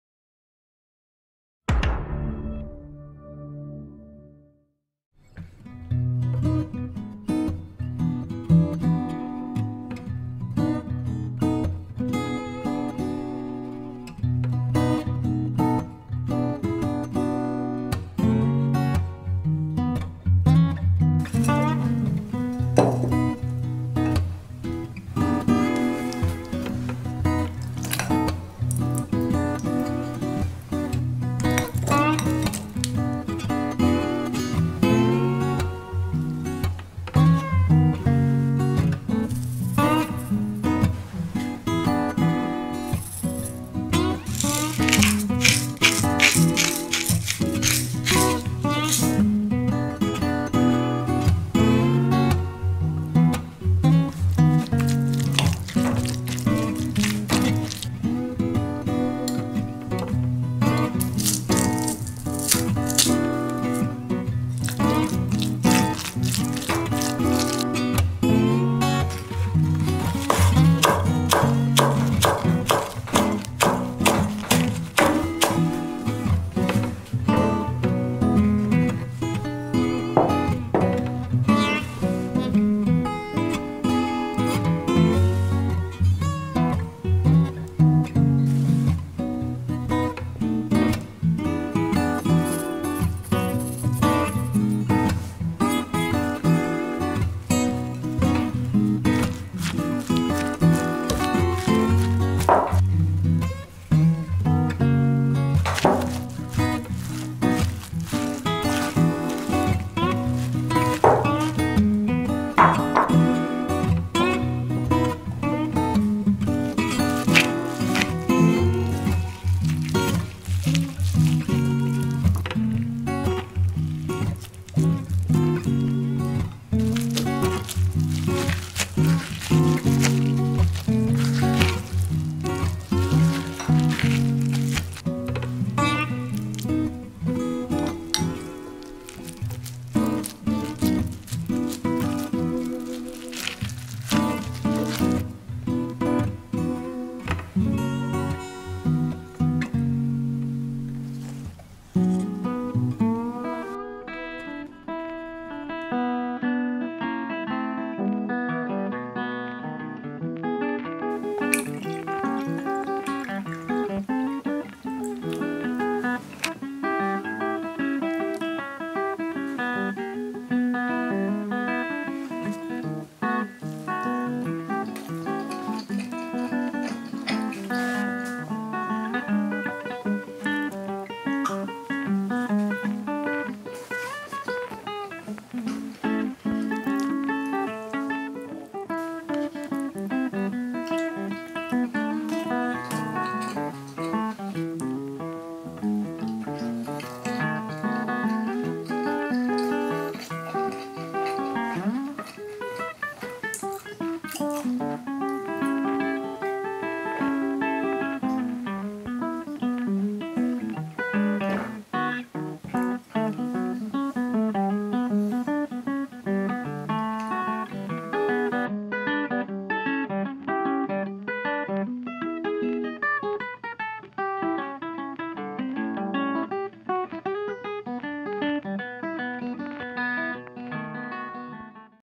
바질 참치 주먹밥 / basil tuna rice ball/how to cook /recipe#65
바질 참치 주먹밥

재료

밥  400g

바질 잎 40g

캔참치 100g

마늘 3알

설탕 1티스푼

마요네즈 2테이블 스푼

올리브유 3테이블 스푼

후추 조금

소금 조금

Basil Tuna Rice Ball

Ingredients

Cooked rice 450g

50 g basil leaves

100 g canned tuna

3 Garlic

1 teaspoon sugar

2-3 tablespoons mayonnaise

2-3 tablespoons olive oil

A little pepper